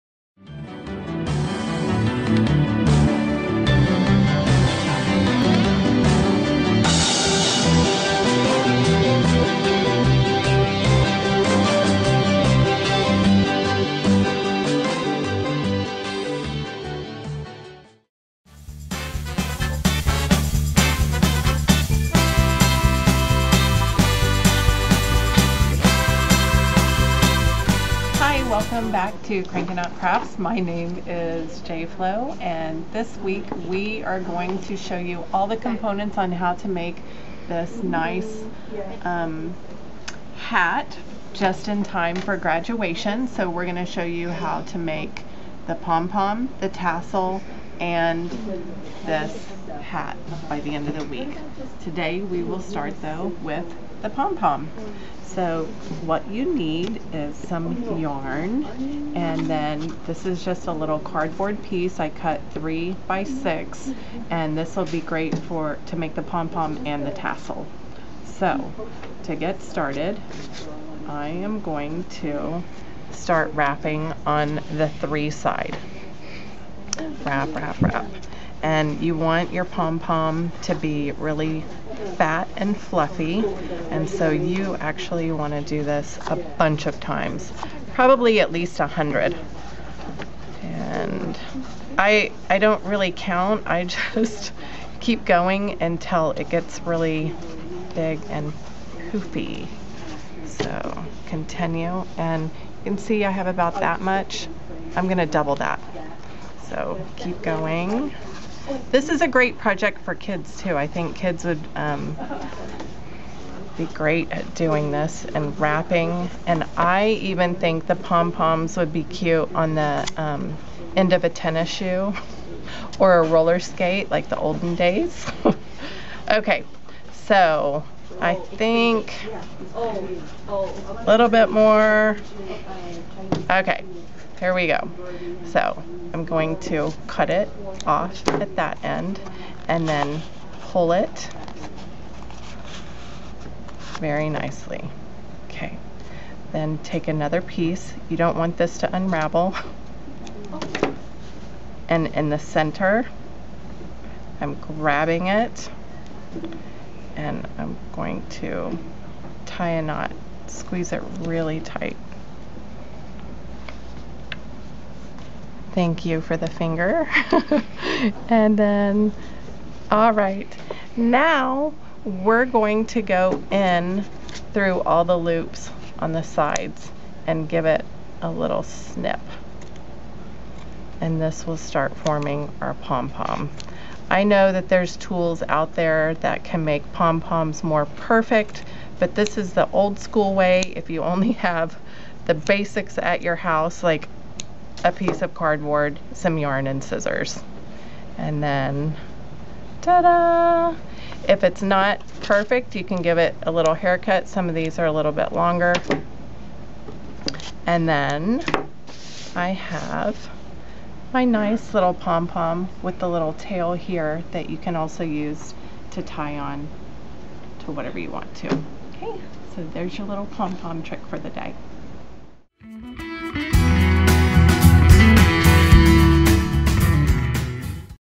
Crankin' Out Crafts -ep236 Grad Cap Box 1 - Pom Pom
Watch how to make a yarn Pom Pom from scratch with a few simple tools.

*For a complete list of supplies and to see more of our projects, visit our Ideas & Inspirations blog

Lei Making supplies available for purchase at all Hawaii Ben Franklin Crafts stores